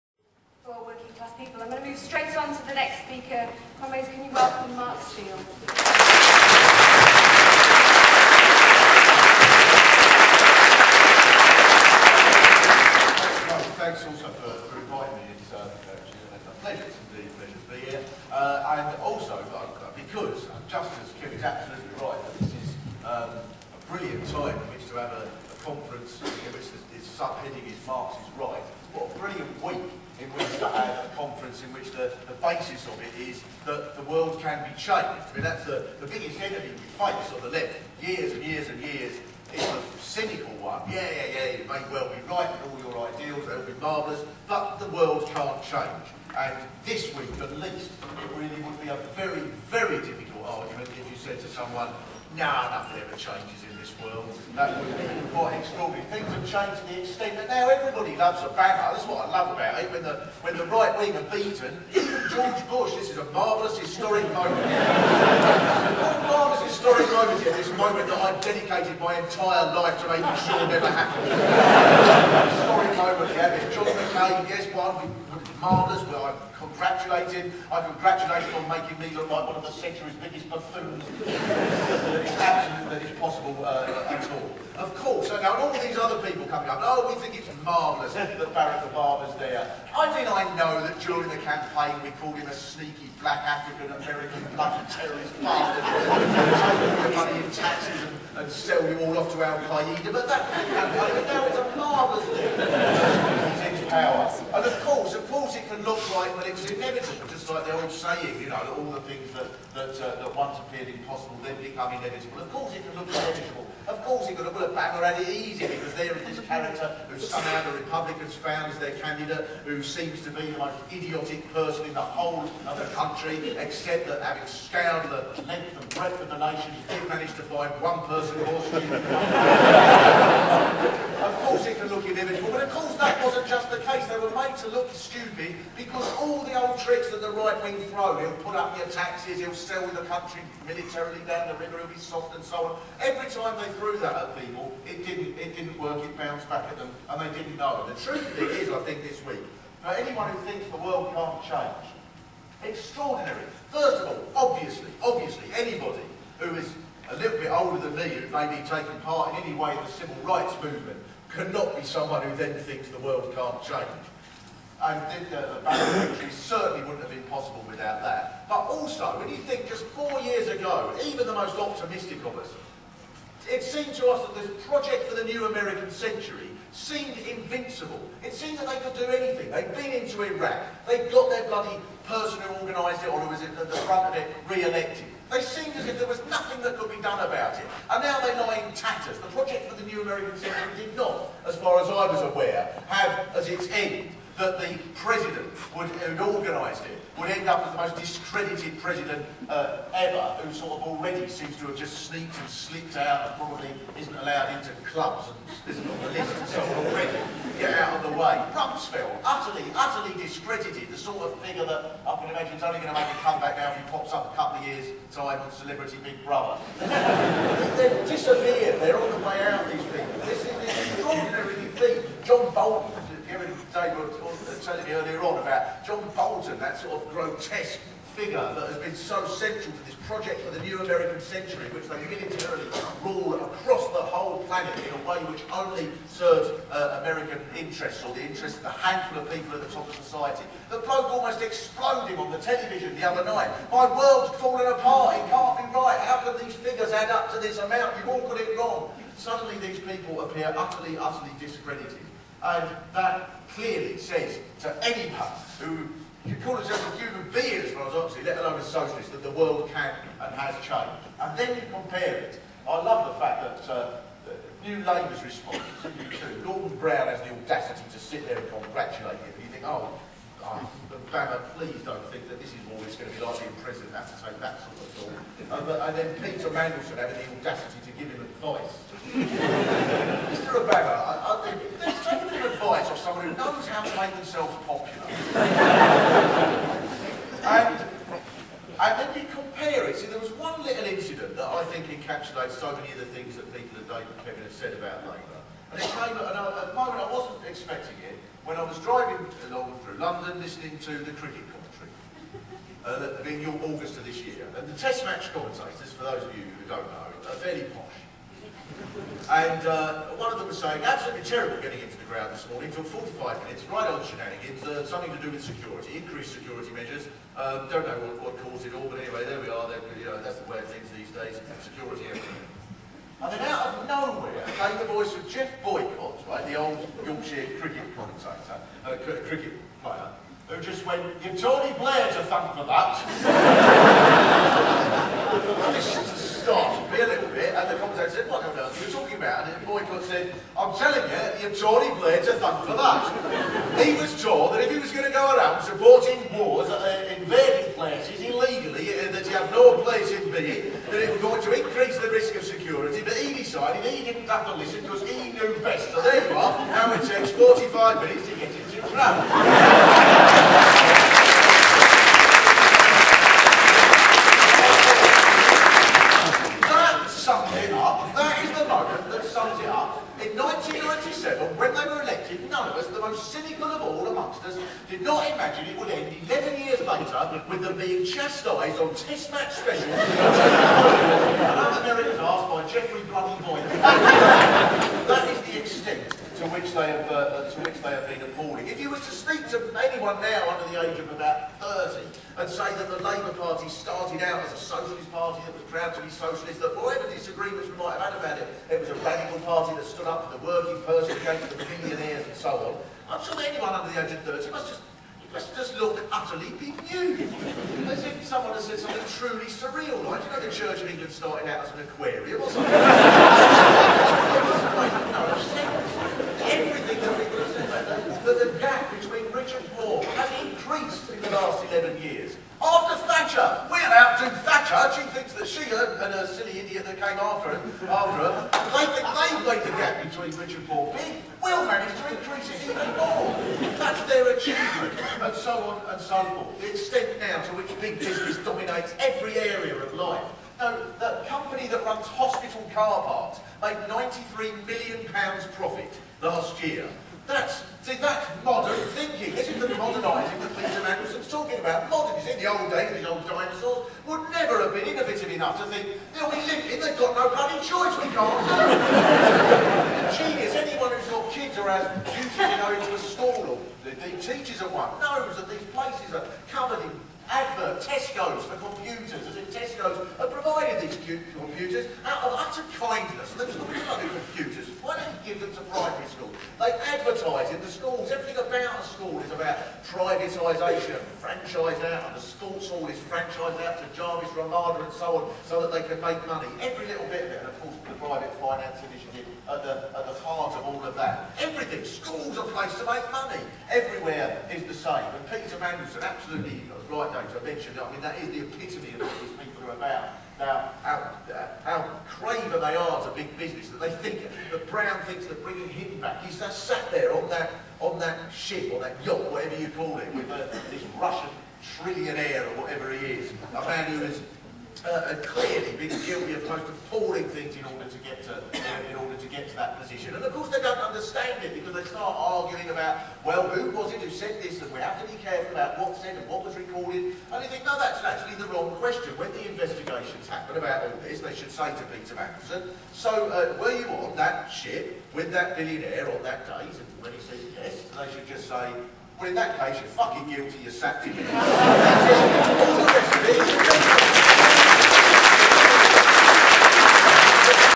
Socialism 2008 London Rally 1/2
Mark Steel addresses the question of the need for a new workers party